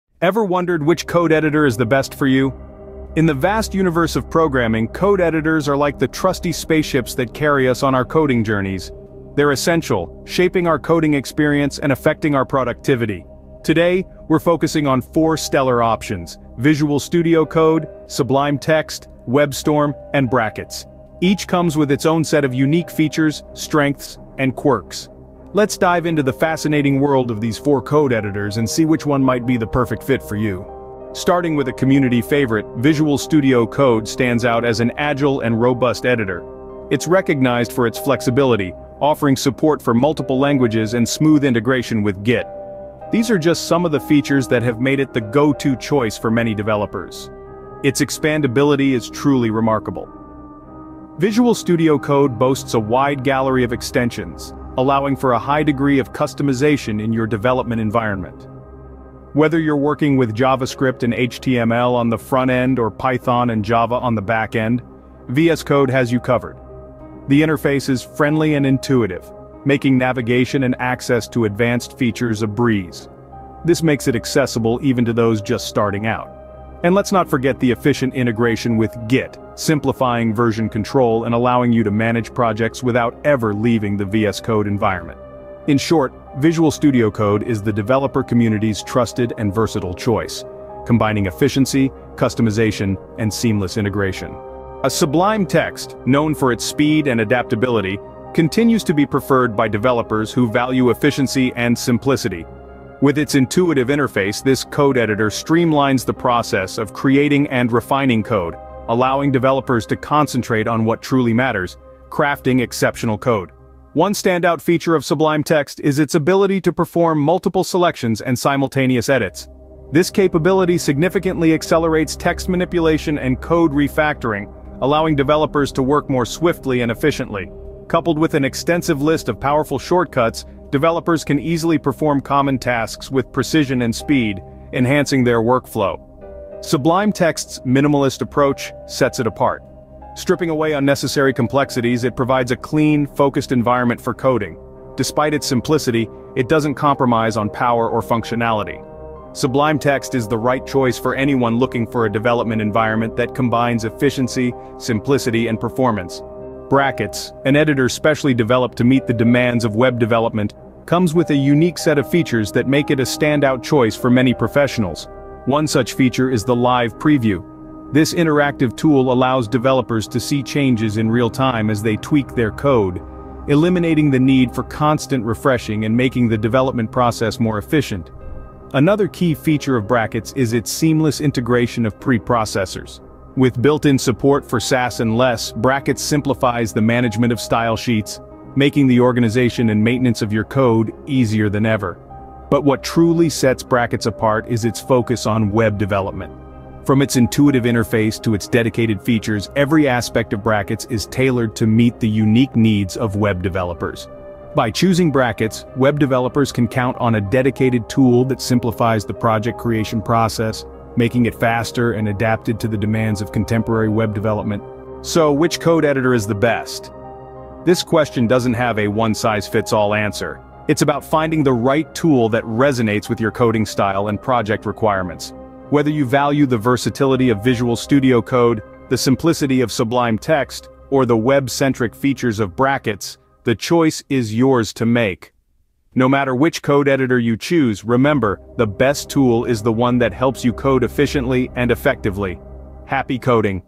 Choosing the BEST Code Editor for 2024
In this video, we delve deep into the fascinating universe of code editors, comparing popular options like VSCode, Sublime Text, WebStorm, and Brackets. Join us as we explore their features, functionalities, and user experiences to help you find the perfect fit for your coding needs. Whether you're a beginner or a seasoned developer, this comparison will provide valuable insights to enhance your coding workflow. Like this video if you found it helpful and share it with your fellow coders!

OUTLINE:
00:00:00
Introduction - The Code Editor Dilemma

00:00:30
Visual Studio Code - The Community's Favorite

00:01:34
Sublime Text - A Model of Efficiency and Simplicity

00:02:36
Brackets - The Web Developer's Friend

00:03:34
Conclusion - Choosing the right Code Editor